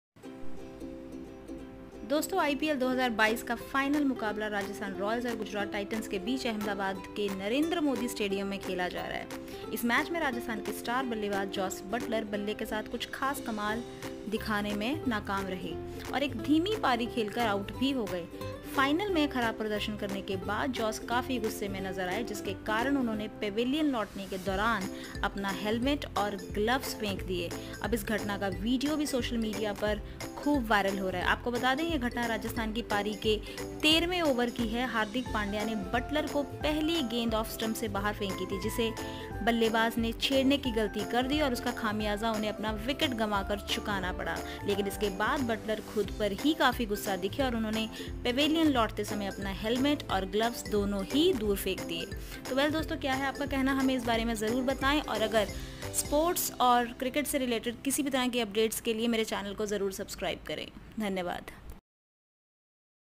Jos Buttler threw helmet & gloves in IPL 2022 final match today video|Jos Buttler batting today
Jos Buttler threw helmet & gloves after dismissal against GT in IPL 2022 final today match video|Jos Buttler wicket today match

Photo Credit:- BCCI/IPL